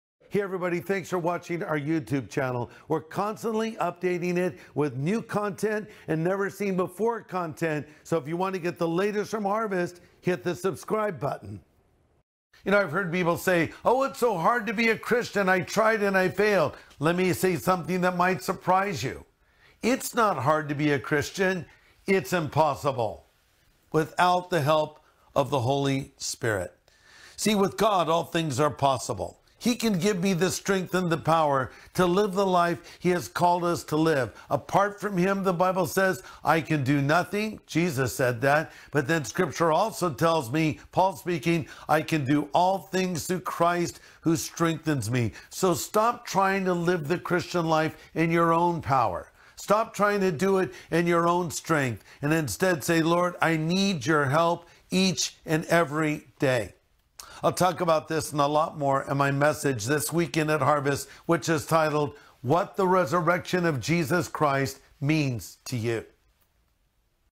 Its Impossible To Be A Christian (With Greg Laurie)
Did you know It's Impossible To Be A Christian? That's right, without the help of the Holy Spirit we can not even hope to follow Jesus!

Support Harvest at Home, help us spread the gospel by going

To get daily devotions delivered right to your phone visit us